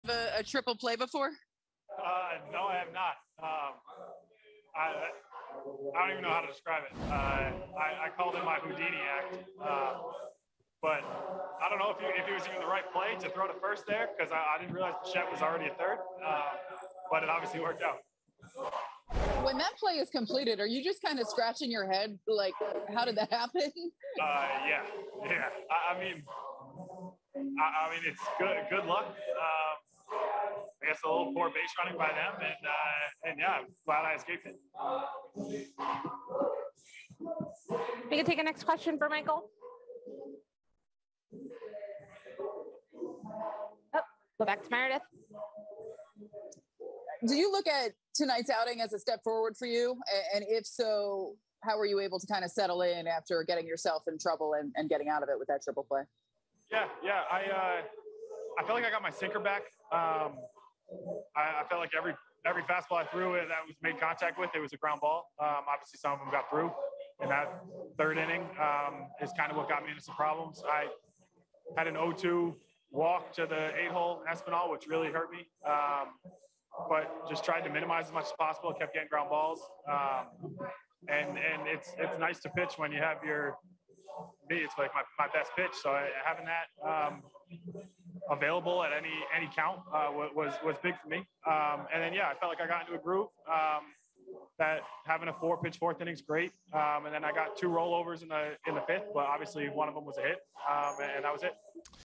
Michael King on his performance in Thursday's 8-4 win over the Blue Jays
Michael King talks the Yankees defense and his performance after Thursday's 8-4 win in Buffalo.